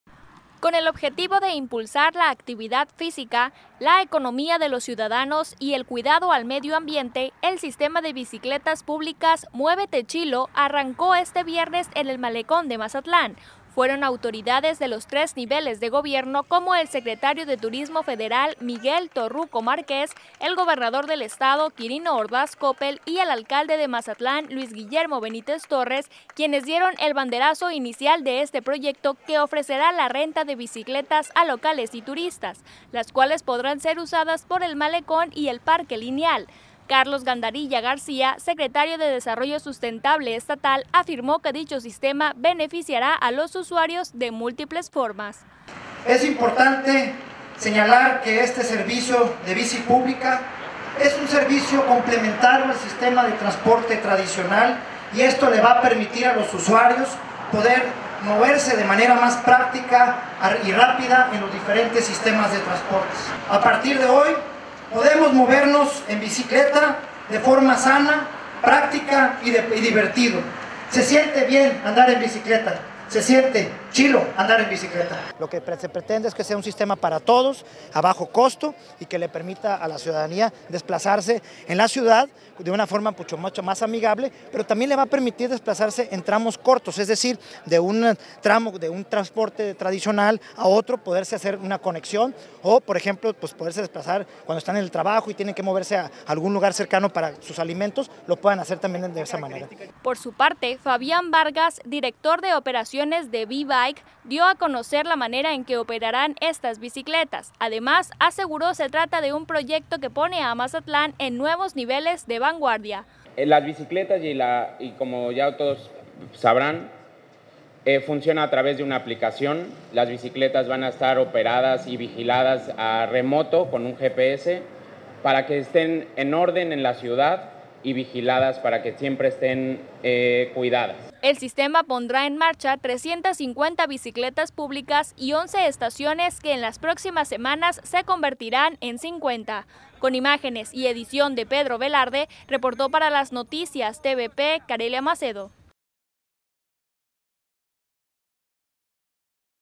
Arranca Sistema de Bicicletas Públicas en Mazatlán
Fueron autoridades de los tres niveles de Gobierno, quienes dieron el bandezaro inicial de este proyecto que ofrecerá la renta de bicicletas a locales y turistas las cuales podrán ser usadas por el malecón y el parque lineal.